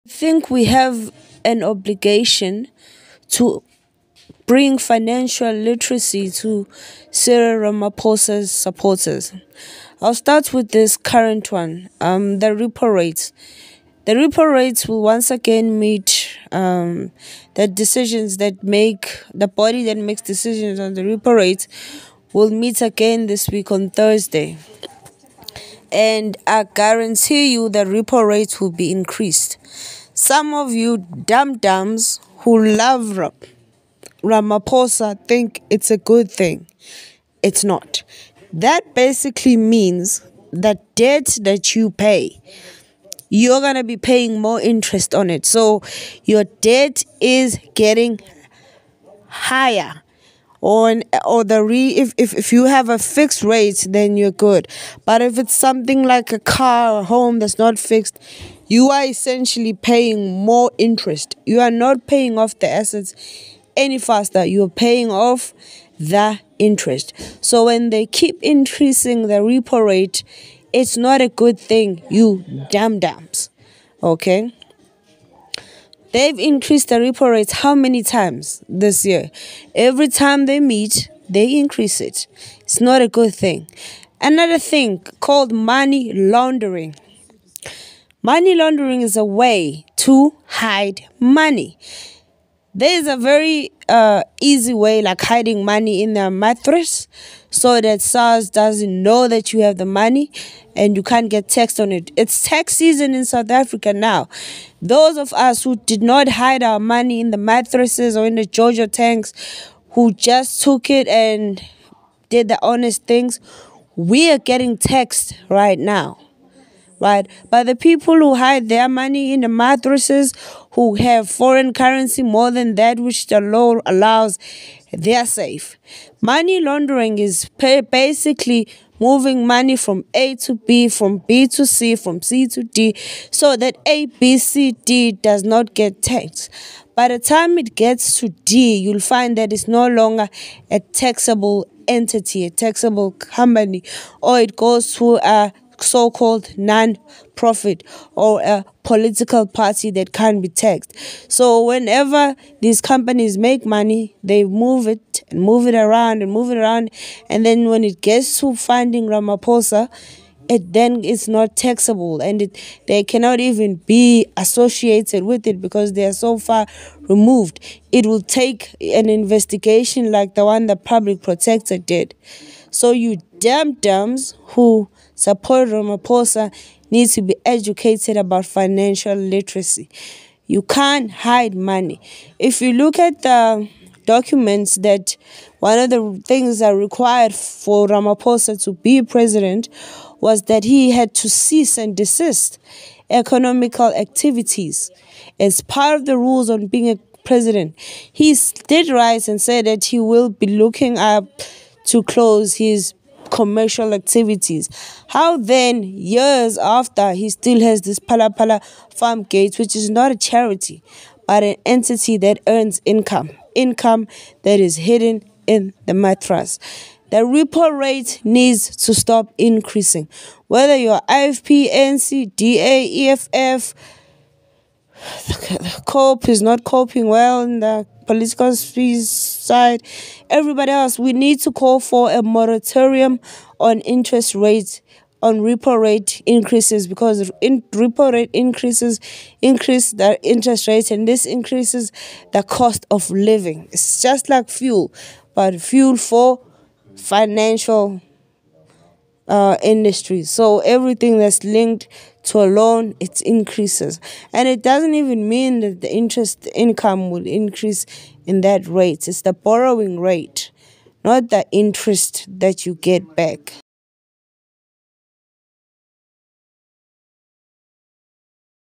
The IDIOT'S guide to supporting Cyril Ramaphosa. Repo rate increases, tax evasion & money laundering
The life expectancy in South Africa has gone down from 70-80 to 60-70 years from Jacob Zuma's term to Sililo(cry) Ramaphosa.

the constant increases in repo rates are adding fuel to the fire. We will be ashes by the time his term ends.